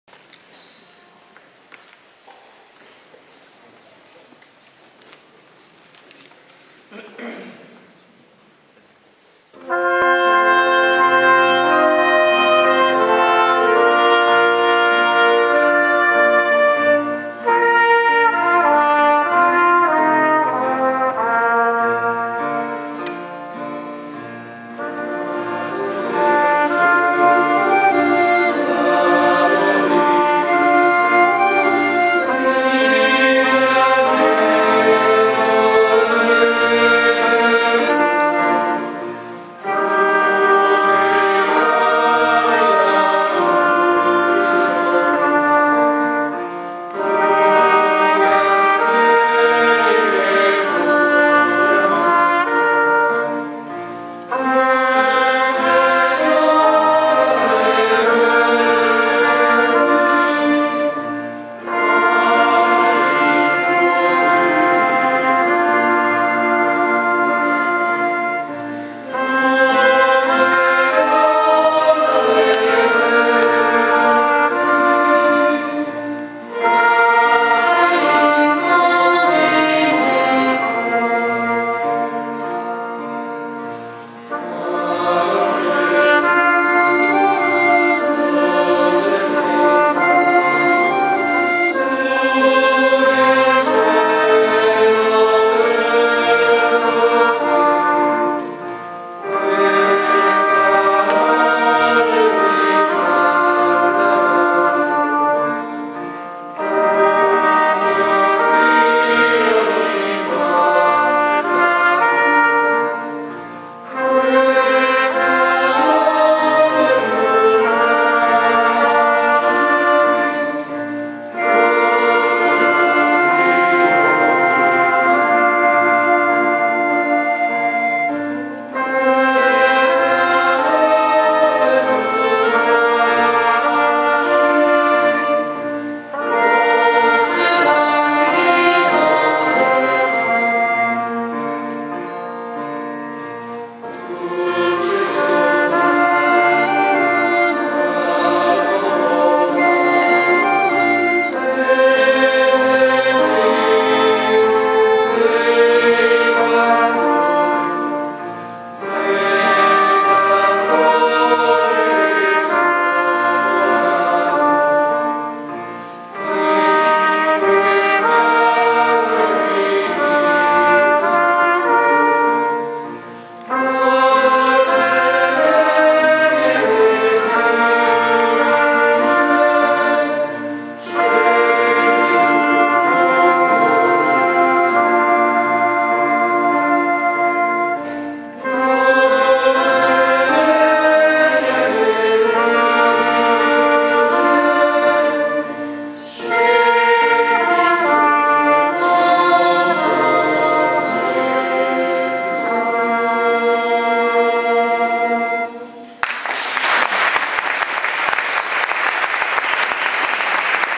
Concerto di canti Natalizi 5 di 16
Concerto di canti Natalizi nella chiesa di San Francesco ad Ottaviano con i maestri Aniello e Giovanni Sepe e il coro Aedi del Borgo